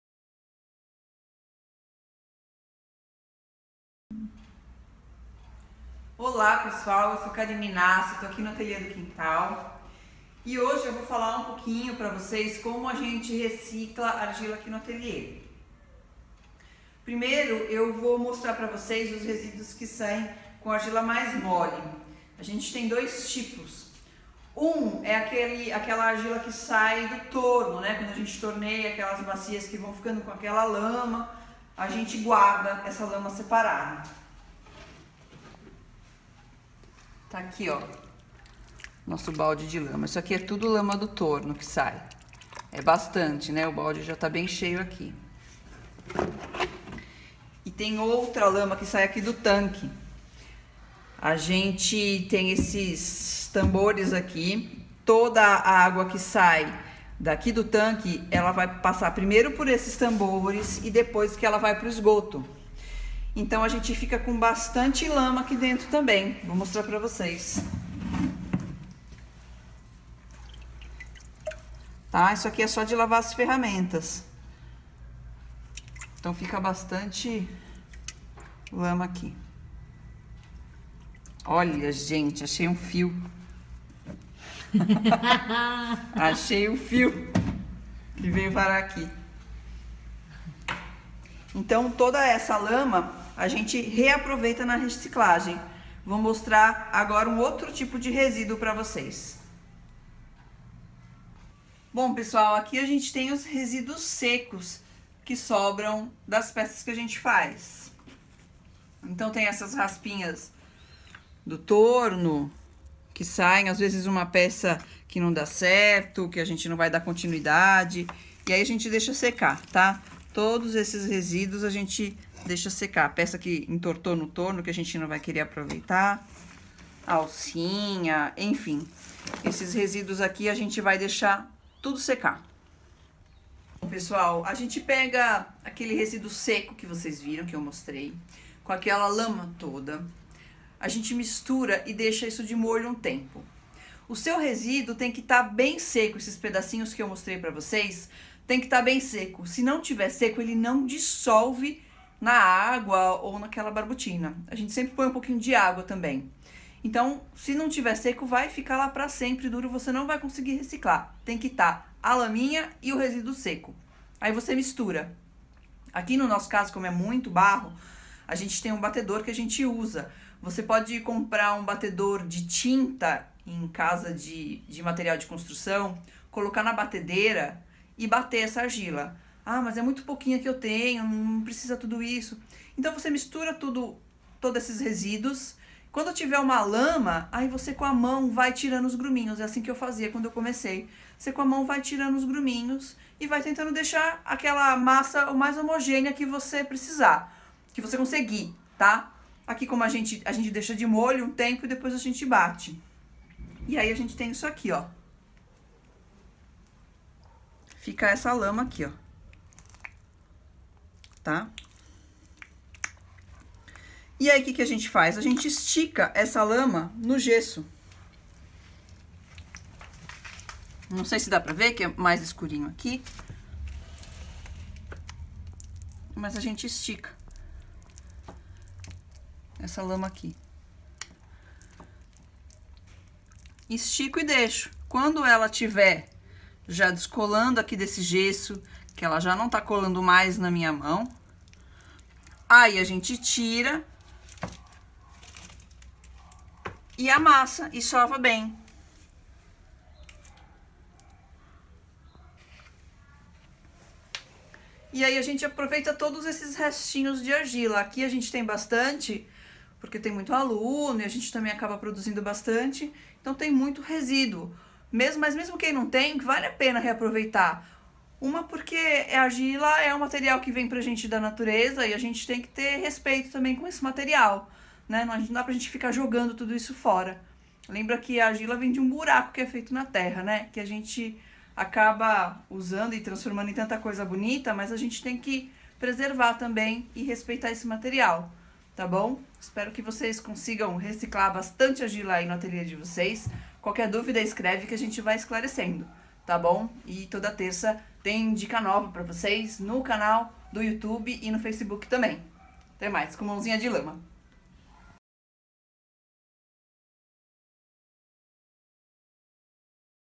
Dica de Cerâmica - Reciclagem de Argila
Hoje é terça, dia de Dica de Cerâmica no Ateliê do Quintal!

Sabe todos os restinhos que sobram de argila, e mais aquela lama toda?
Então! Vamos reciclar tuuuudo!

Todos os vídeos estão disponíveis no site também!

E em nosso canal no YouTube!